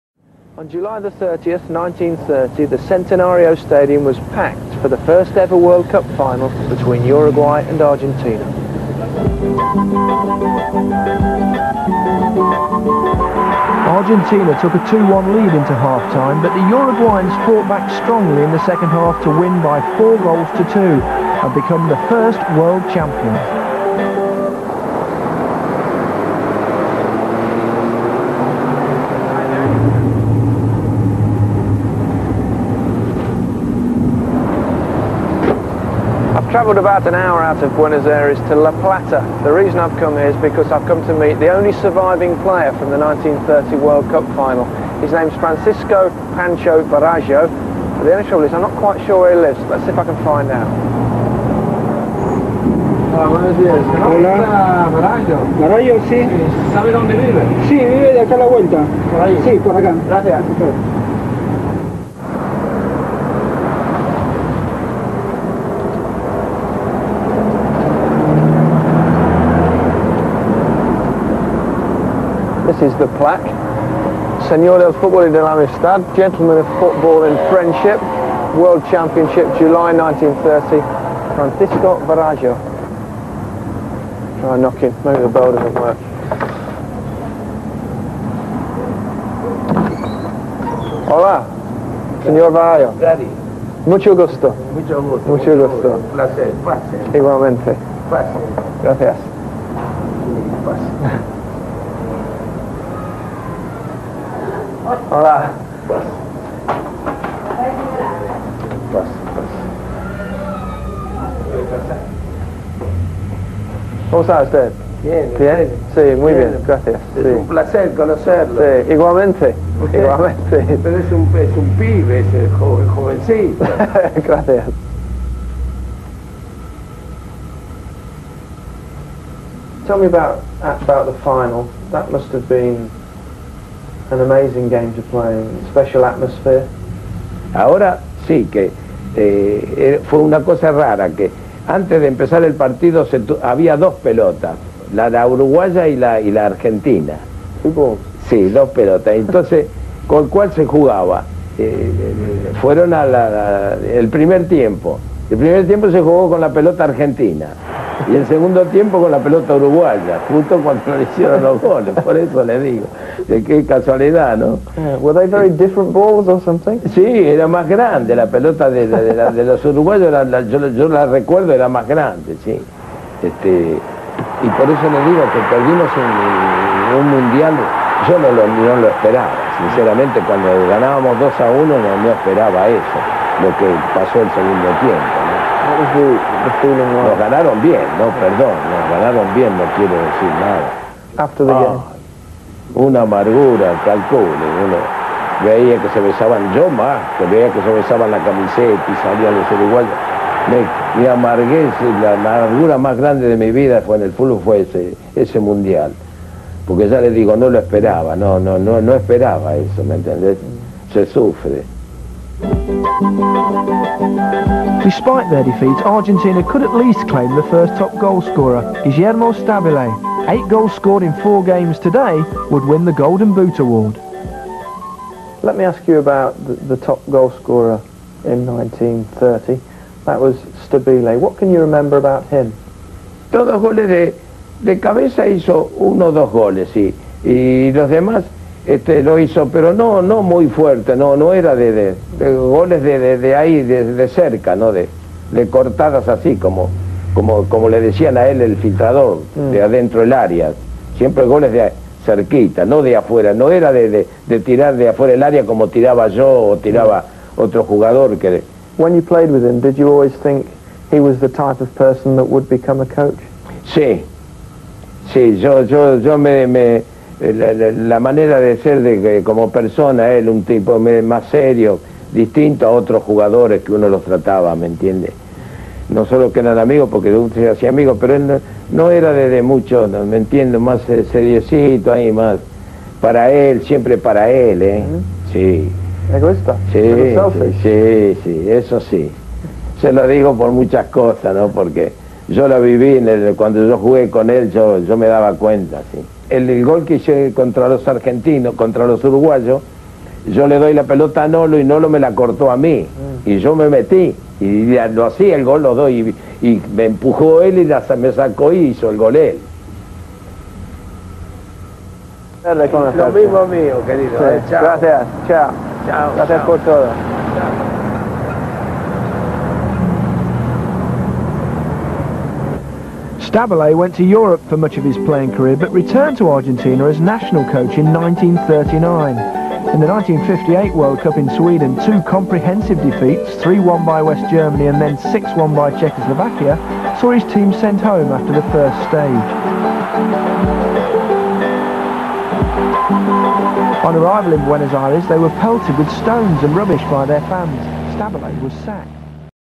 Stábile - Artilheiro da Copa do mundo de 1930
Entrevista com o ex-jogador argentino Varallo. Em 1998, ele era o único jogador presente à primeira final de Copa do Mundo (1930), que estava vivo. Ele fala sobre Guillermo Stábile, artilheiro da Copa do Mundo de 1930. O entrevistador é Gary Lineker, artilheiro da Copa de 1986. Imagens: SPORTV.